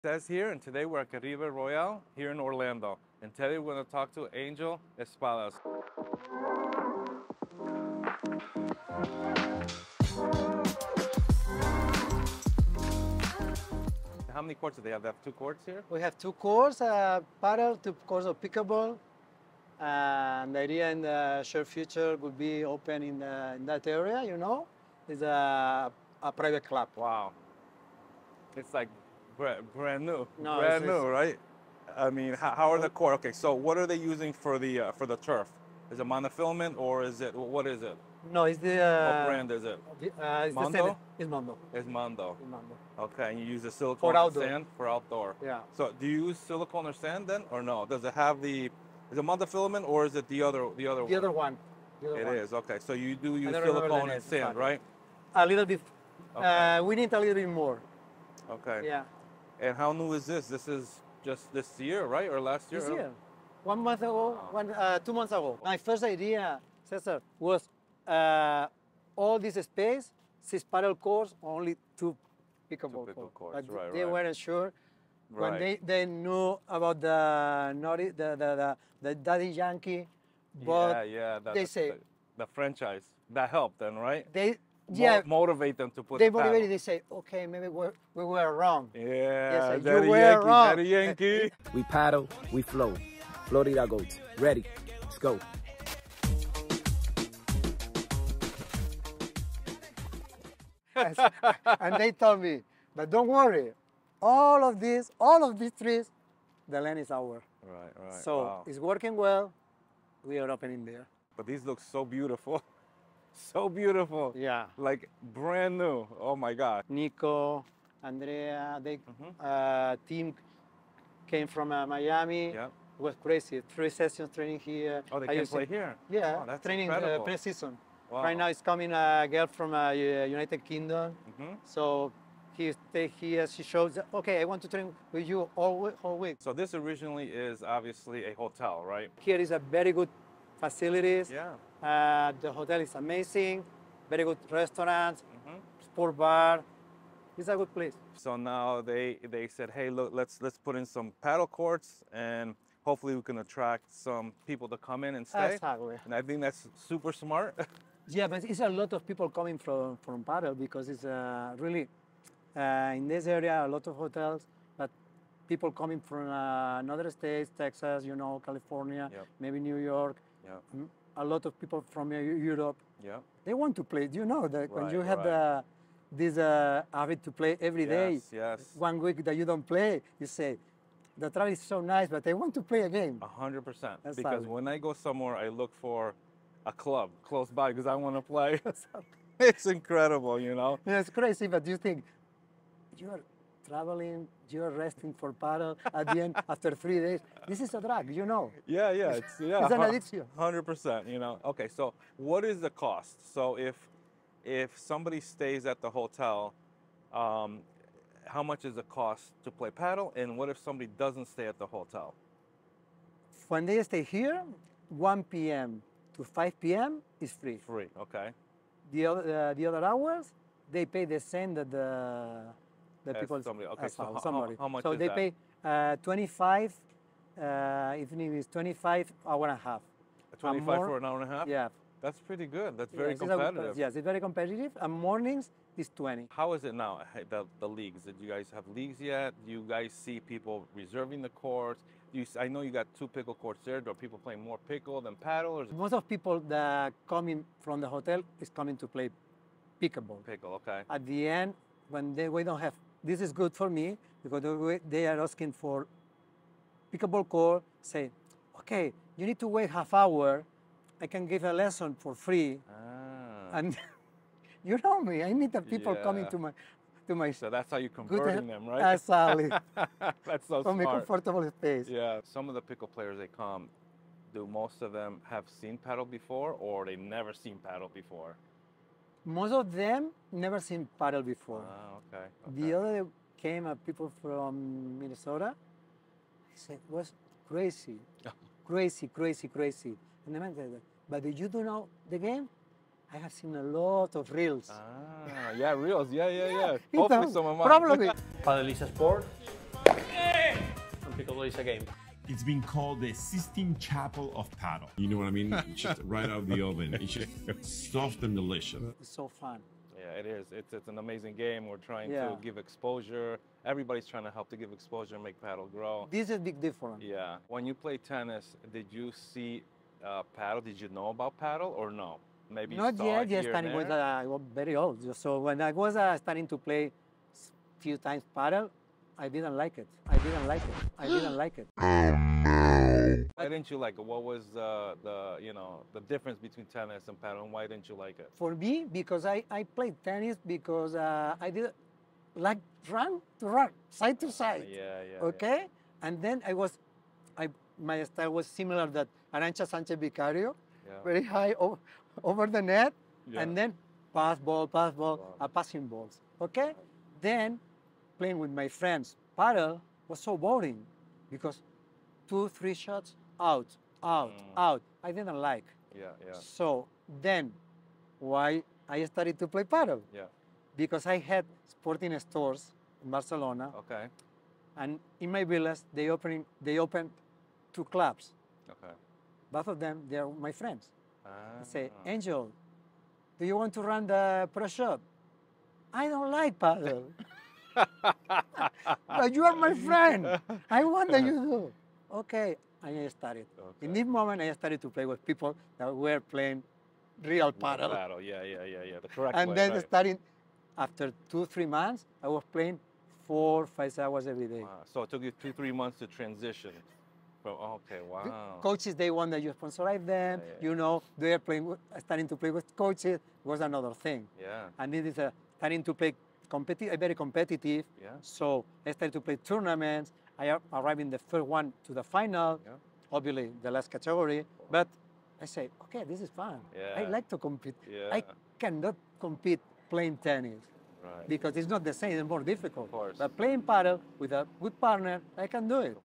The Padel Paradise Hidden in a Luxury Resort!
Join us as Ángel Espadas, a once tennis devotee turned Padel maestro, takes us on an exclusive tour through a Padel paradise nestled within a luxury resort, while also revealing the transformation of his skepticism into a profound love for the sport. Dive into the heart of luxury and passion, where the hidden gems of Padel await to convert even the most doubtful into believers.

Extended Interview with Angel Espadas Streaming on all Podcast Platforms

In this captivating episode from Padel Smash Academy, we dive deep into the world of Padel with an exclusive interview featuring Ángel Espadas, the esteemed coach of the US Padel National Team and a fervent aficionado of the sport. With his dual roles as a professional Padel coach and the director of sports at the luxurious Caribe Royale, Ángel takes our host, César, on an enlightening tour of the resort's brand-new Padel courts, unveiling the elegance and sophistication that define the venue. Beyond the glitz, Ángel shares his intriguing journey from tennis to Padel, revealing how his initial skepticism about Padel transformed into a passionate commitment that has shaped his career. This episode is a treasure trove for anyone eager to explore the intricacies of Padel coaching, gain insights into the sport's growth, and discover how luxury and leisure intertwine with the competitive spirit of Padel at one of the most exquisite resorts. Whether you're a seasoned player, a sports enthusiast, or simply curious about the evolving landscape of racquet sports, this interview promises to enlighten, entertain, and inspire.

This video is powered by Racket Central